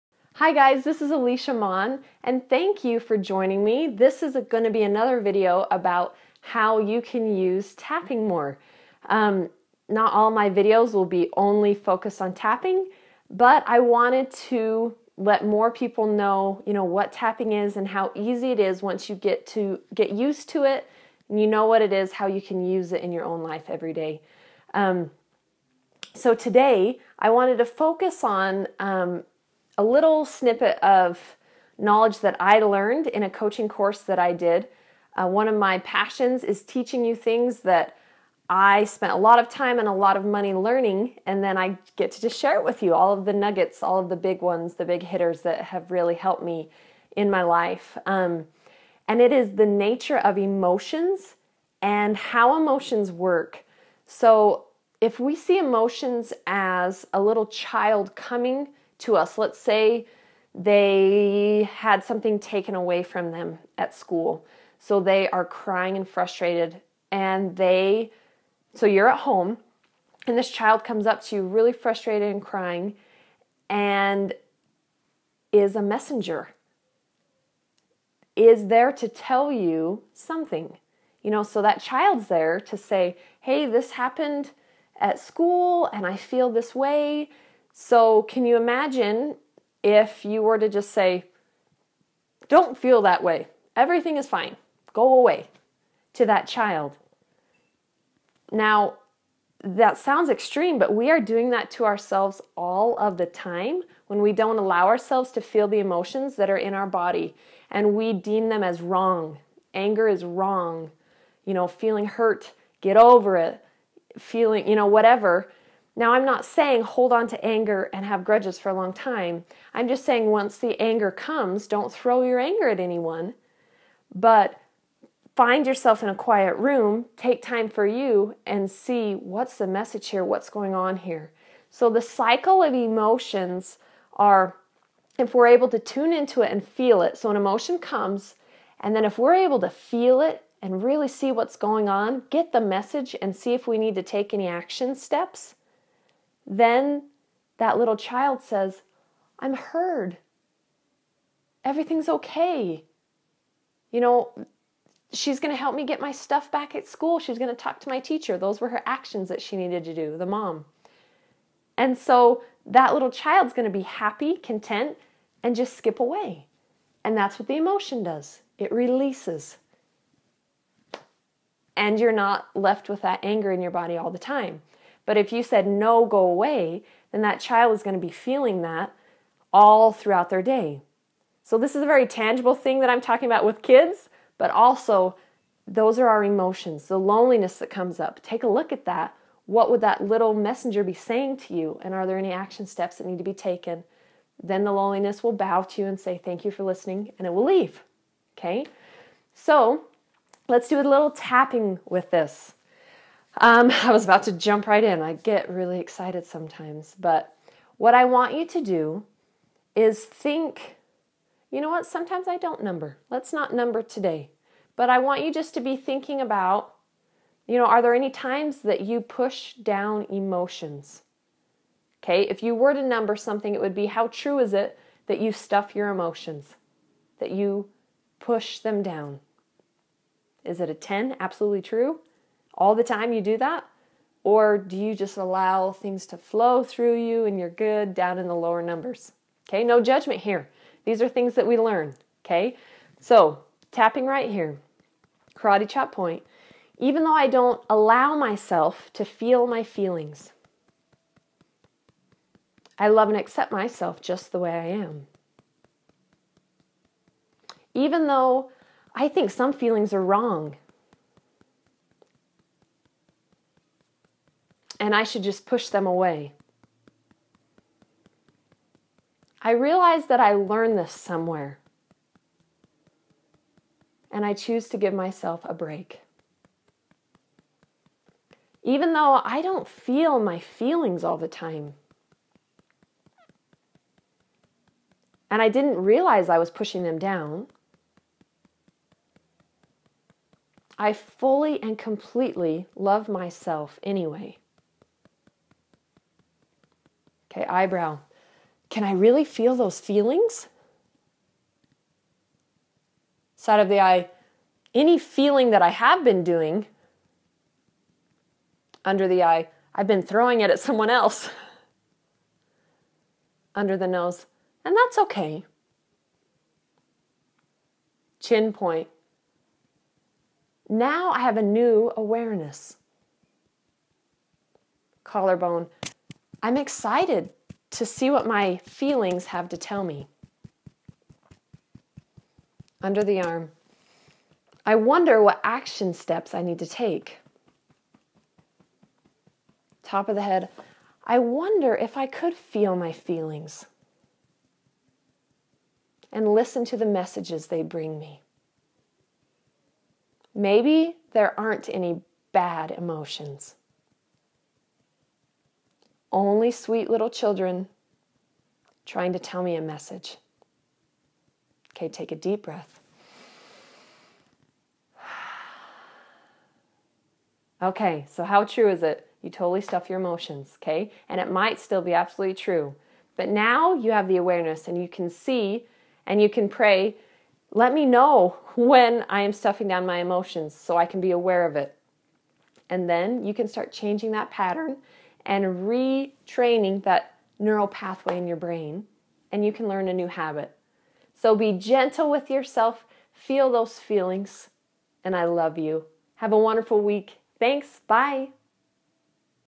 Tapping Fully Feel Emotions
It's a very freeing concept to see emotions as messengers and that they all can be beneficial.